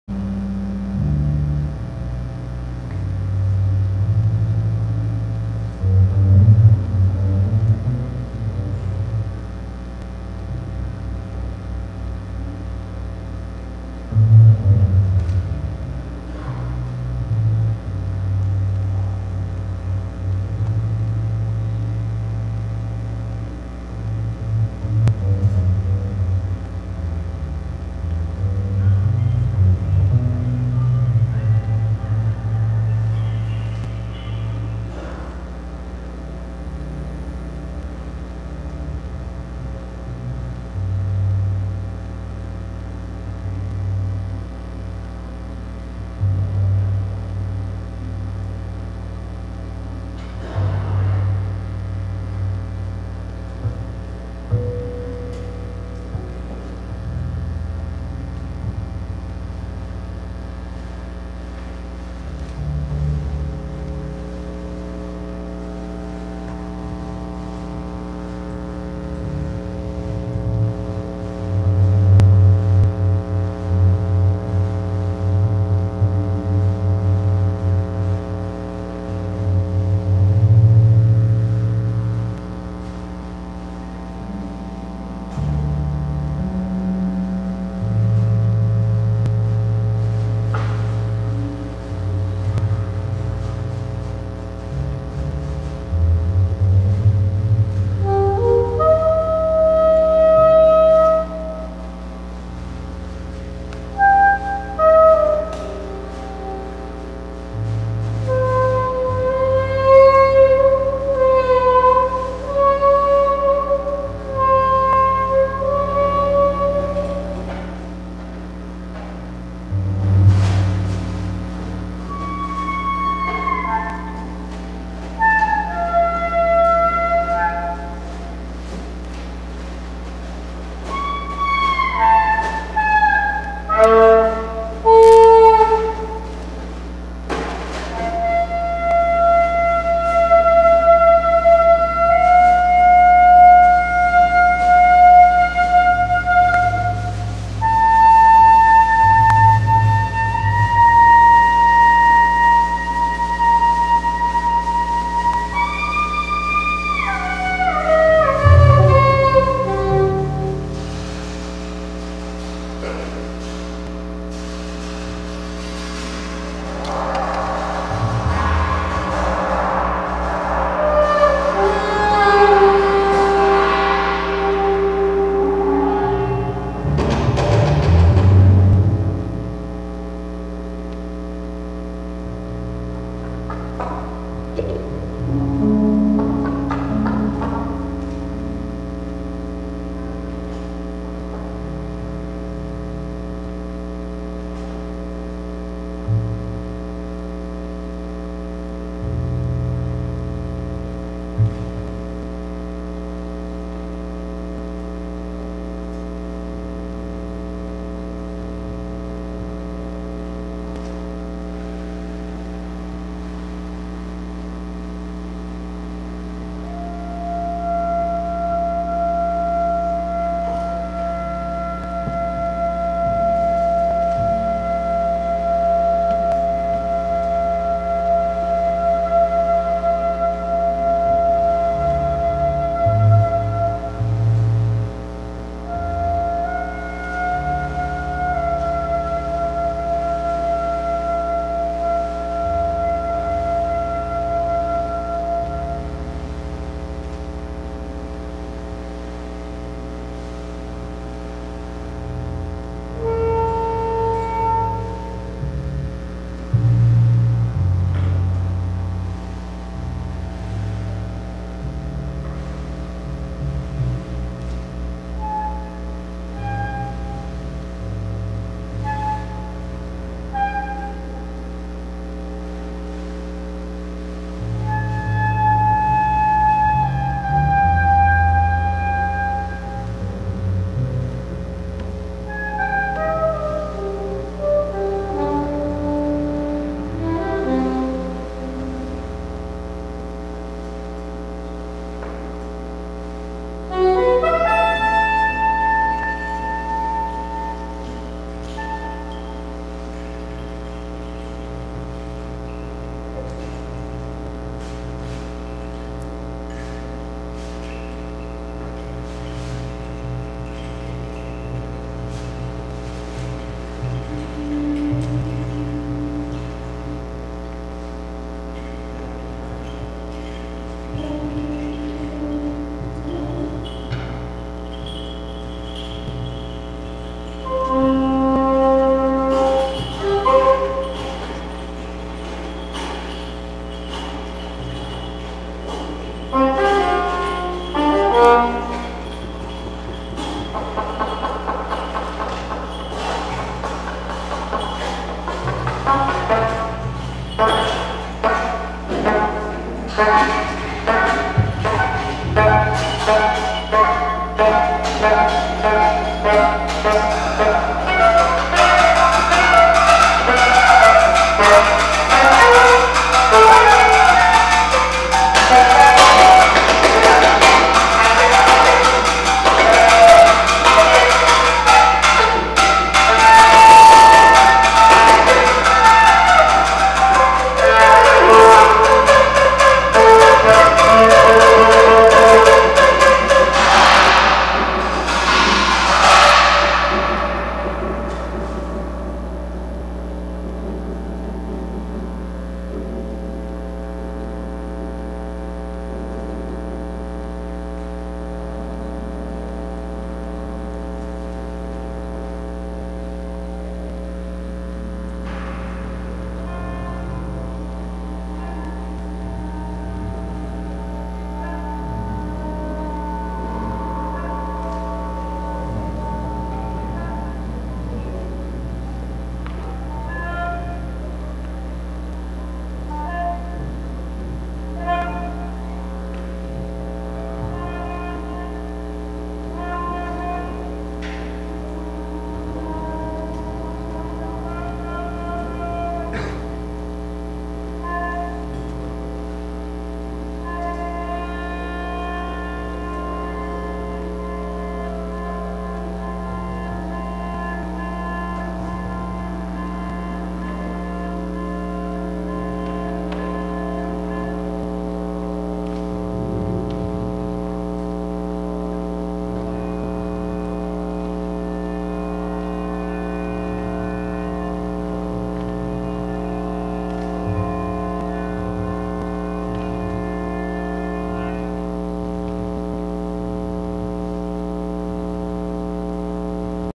SAFMOD (2) 08/17/07
SAFMOD performing in the All Go Signs event at Cleveland Public Theatre.